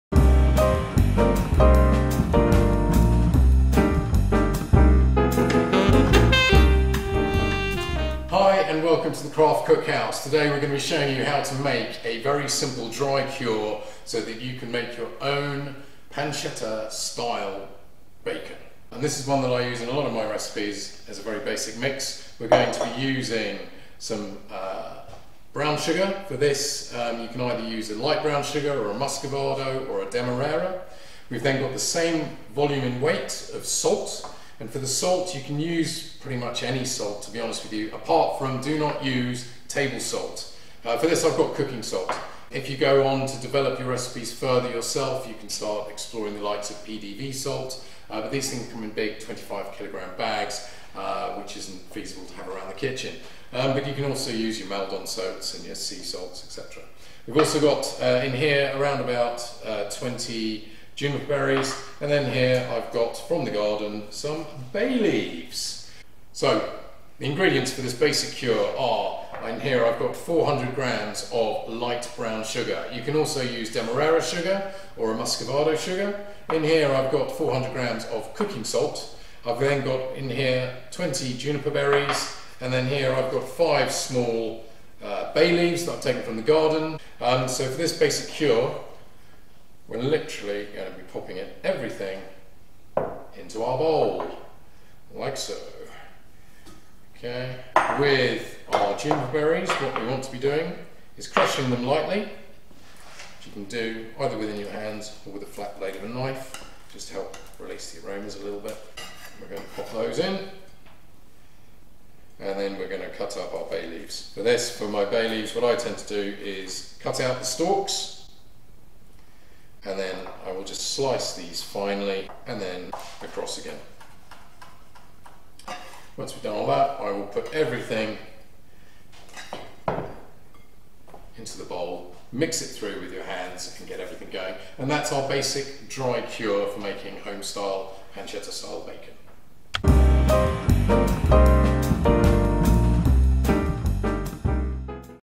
Dry Cure Recipe for Homemade Bacon Recipes [2021]
A simple dry cure so you can make English back bacon, Canadian bacon, or American bacon at home. Just follow this simple guide and the recipes on our website for a simple homemade, additive free bacon.